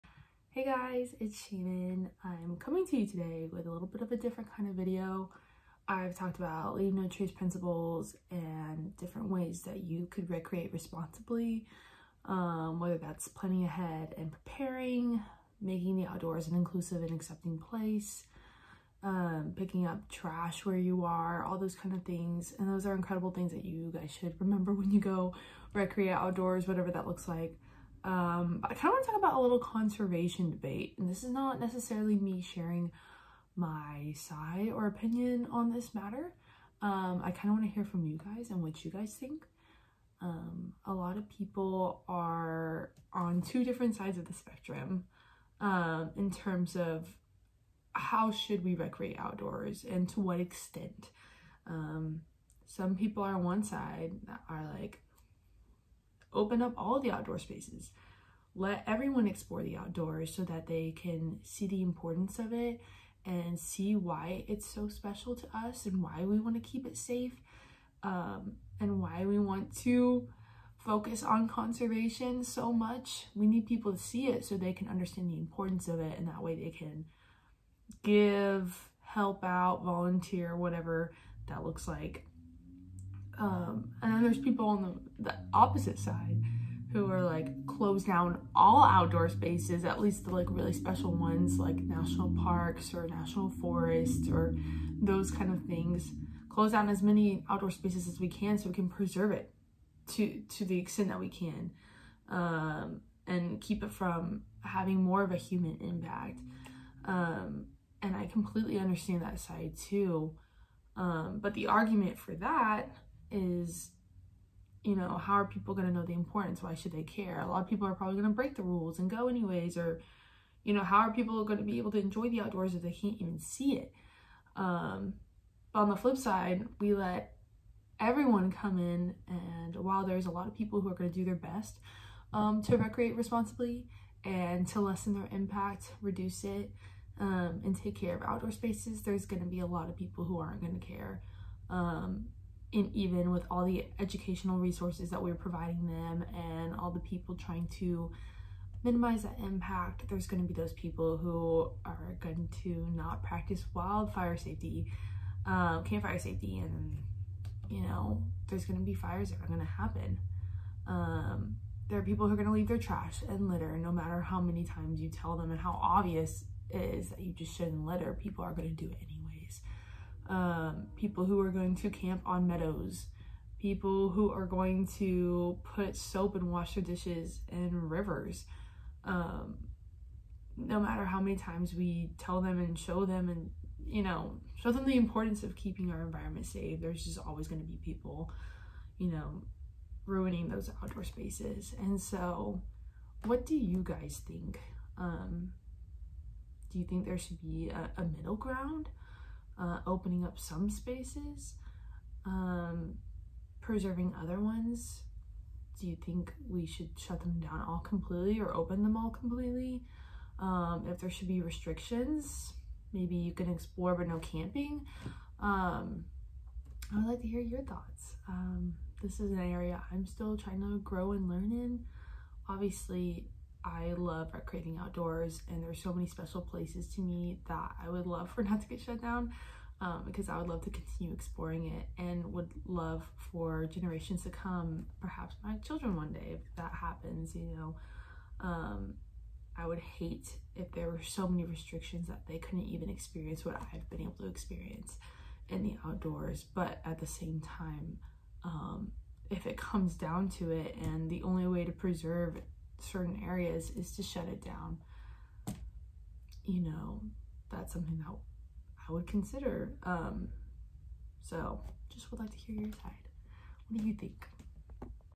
The Conservation Debate - Outdoor Recreation
A popular conservation debate — allow everyone to recreate in all outdoor spaces? Or place strict or even full restrictions on outdoor recreation? How does each side affect the outdoors…and us? 😄